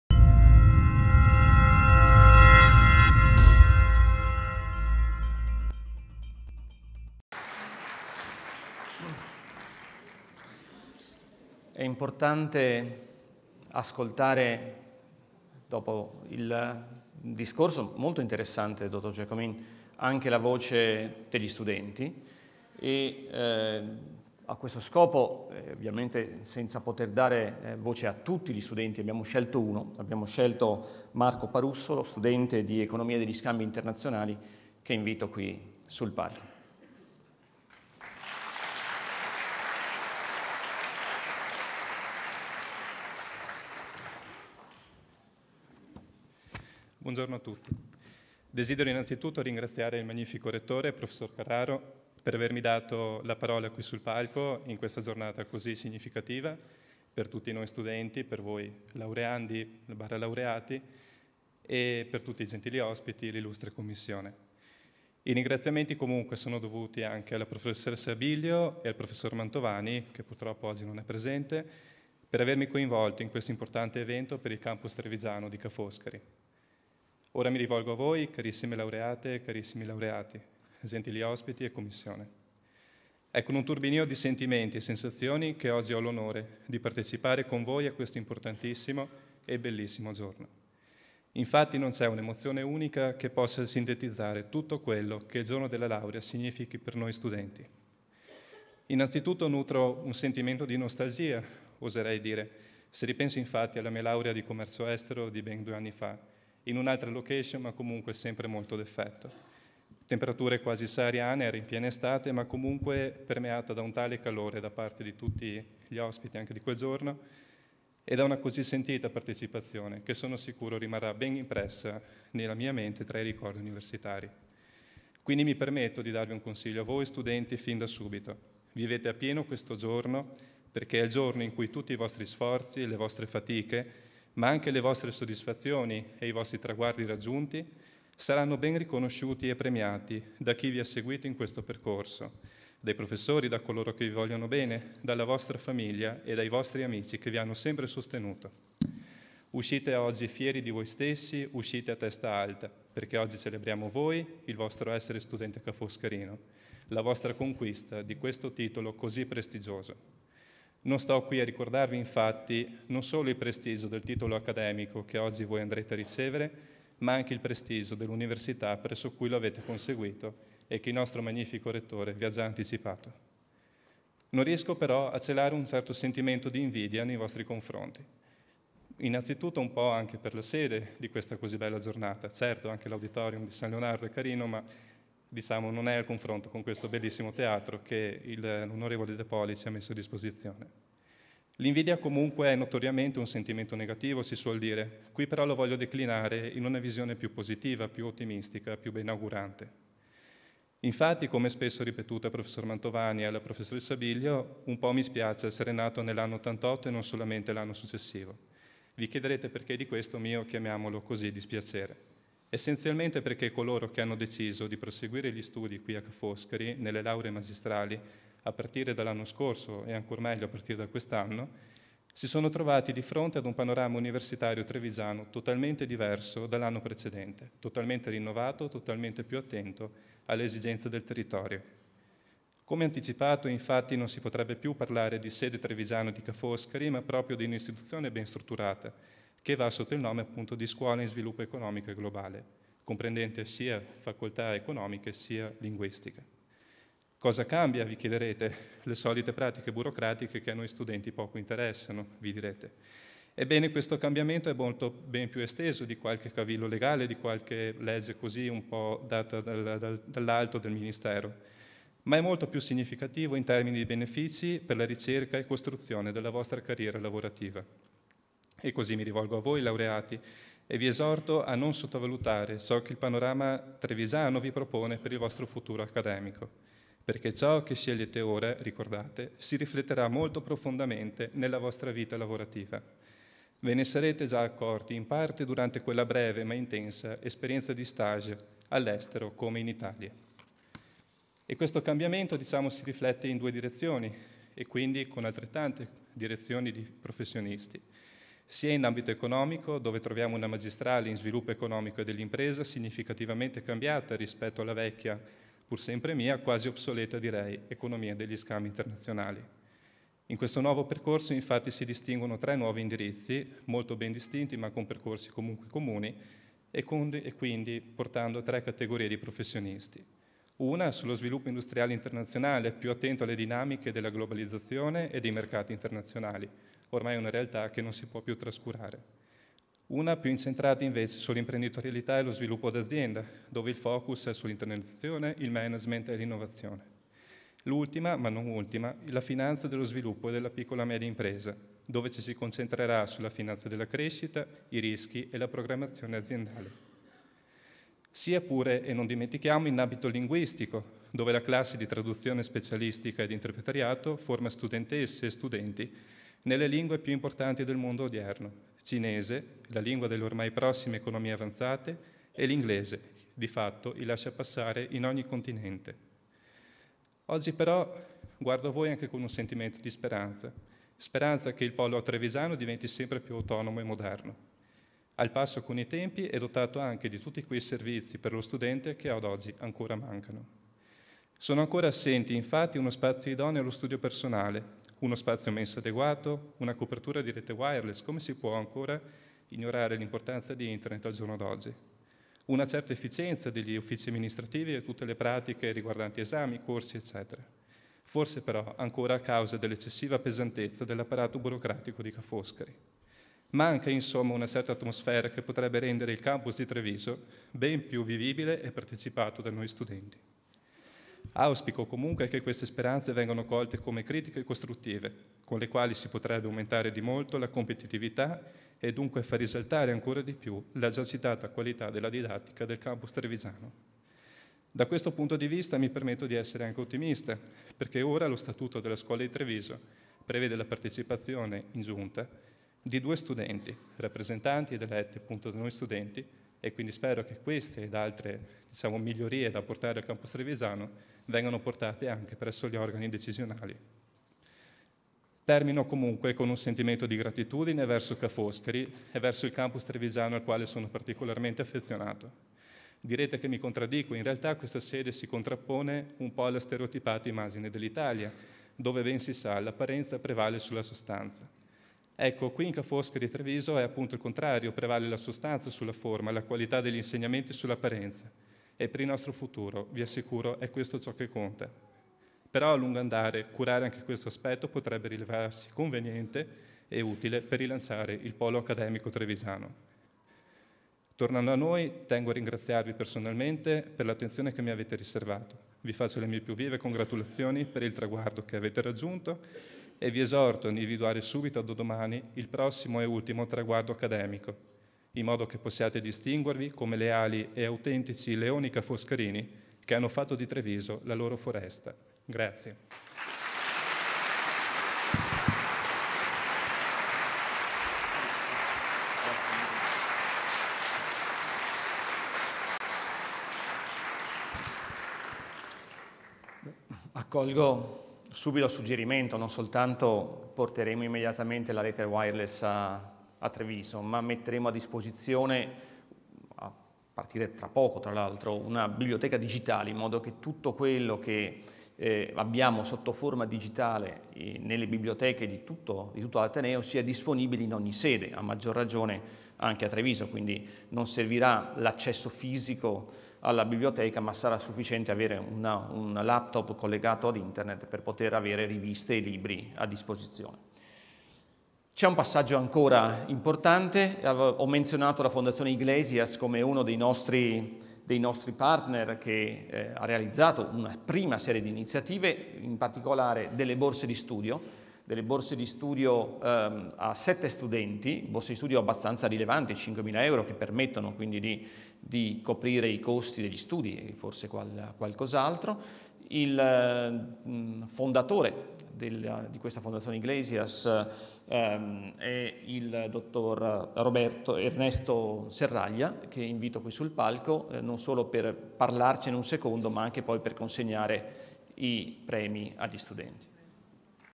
Giorno della Laurea - Treviso marzo 2012 - 4/6
Teatro Comunale "Mario Del Monaco" di Treviso la cerimonia per la consegna dei diplomi per le lauree dei corsi con sede a Treviso dell'Università Ca' Foscari di Venezia, per la sessione straordinaria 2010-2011.
Alla cerimonia hanno preso parte il Rettore di Ca' Foscari Carlo Carraro, Dino De Poli presidente di Fondazione Cassamarca, il direttore della Scuola in Sviluppo Economico Globale Monica Billio e Francesco Giacomin, Presidente della Fondazione la "Fornace dell'innovazione" e componente del Consiglio d'amministrazione di Unicredit.